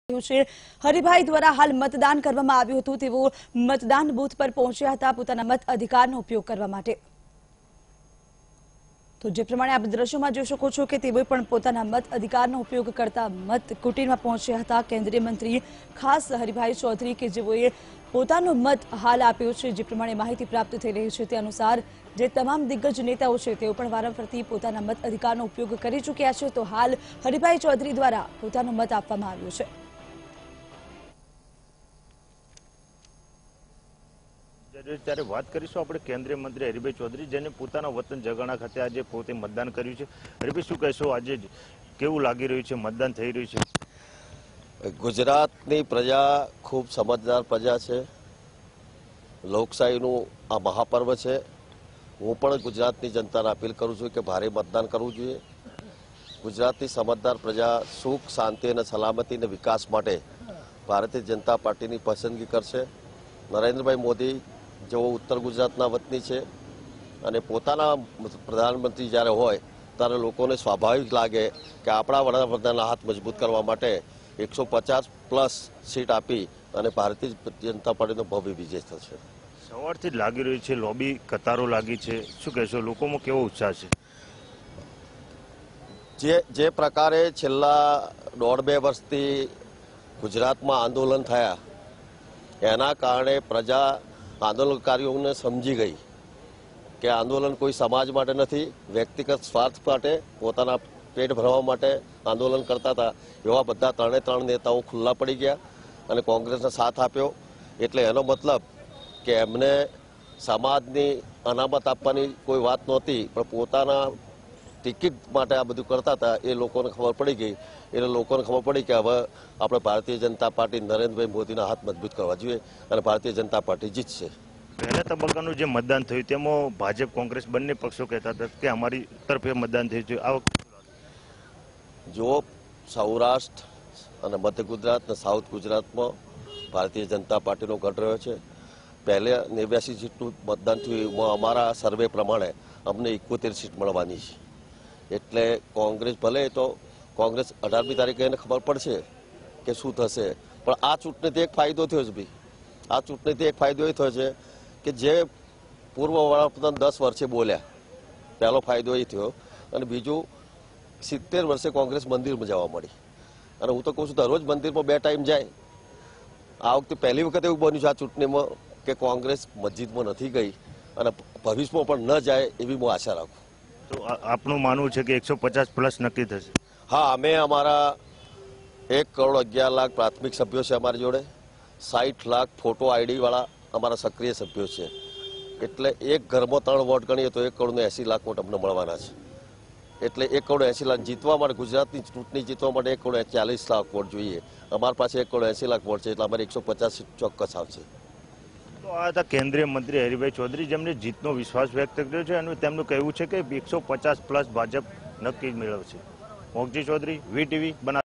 2nd Phase Elections: Union Minister Haribhai Chaudhary casts his pious vote | Vtv News
2nd Phase Gujarat Elections: Union Minister Haribhai Chaudhary casts his pious vote